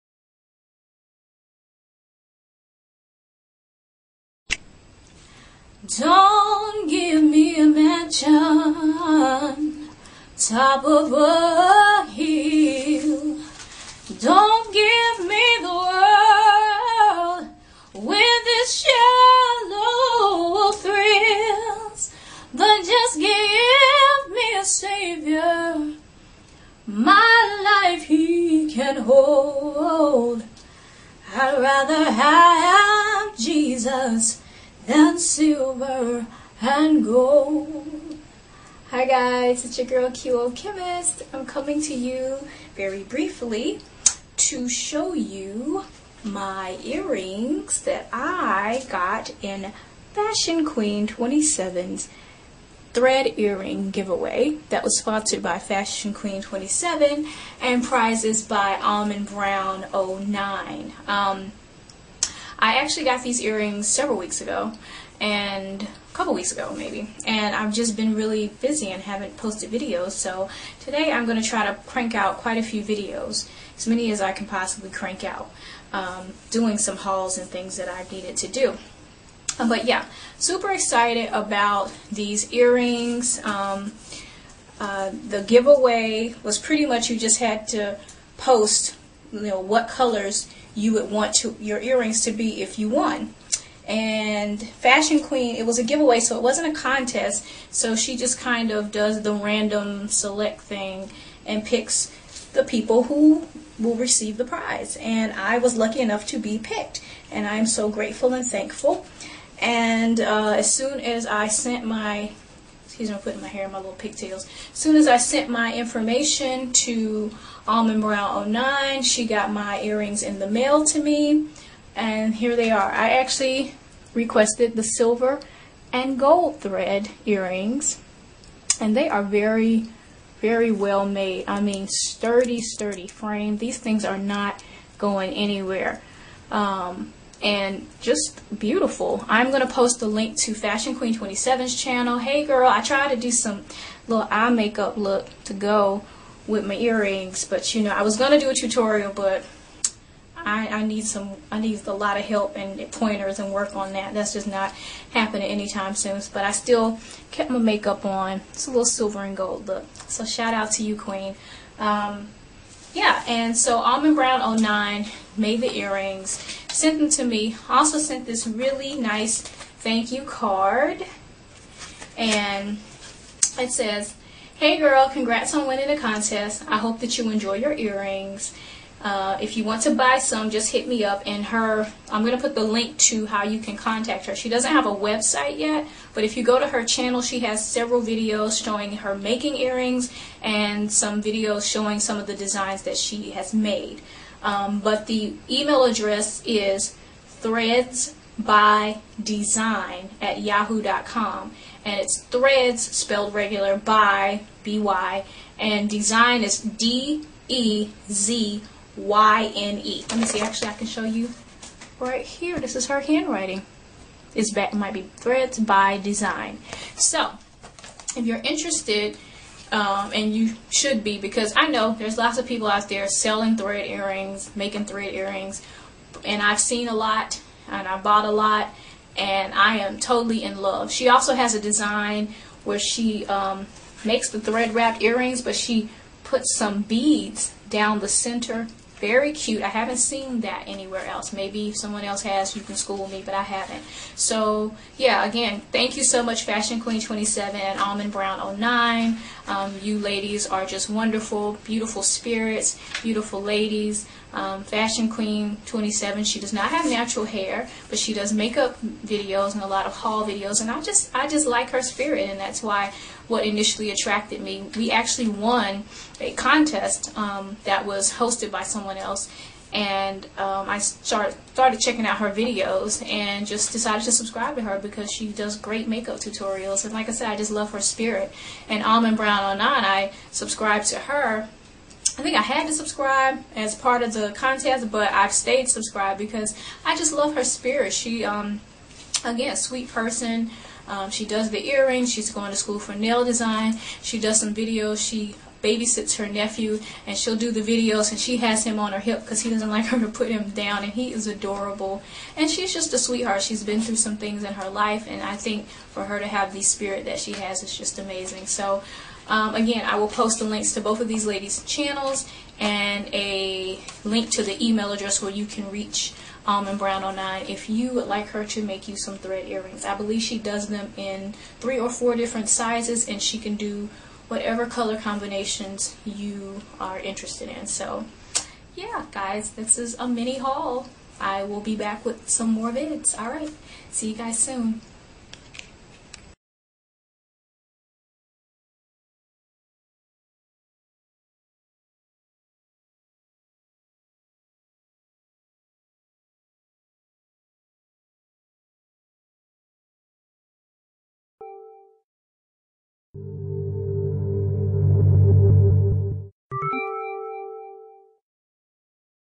Silver And Gold- Thanks Ladies!!
This is an overdue vid thanking Phasionqueen27 and Almondbrown09 for selecting me in the Thread Earring Giveaway... I love my silver and gold earrings ladies :)